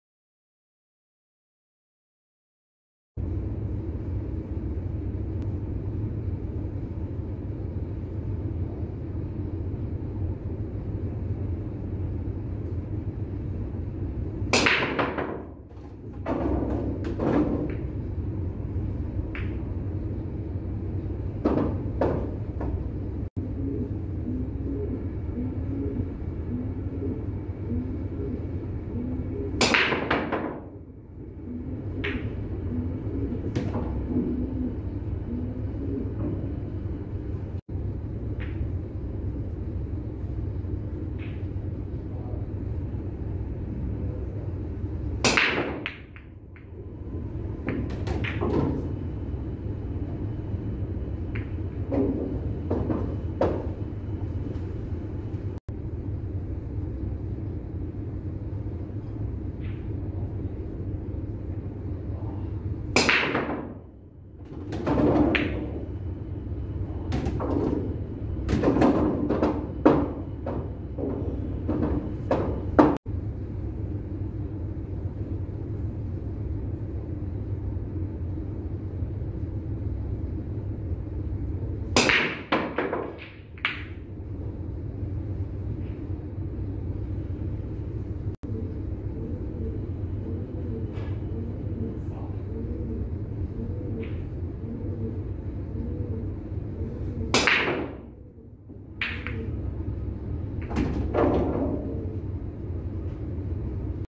More breaks with KAMUI SAI Control Break tip
The KAMUI SAI is installed on a Cuetec WCT break cue.
10-ball breaks: 0:00 - 1:12
8-ball breaks: 1:14 -1:45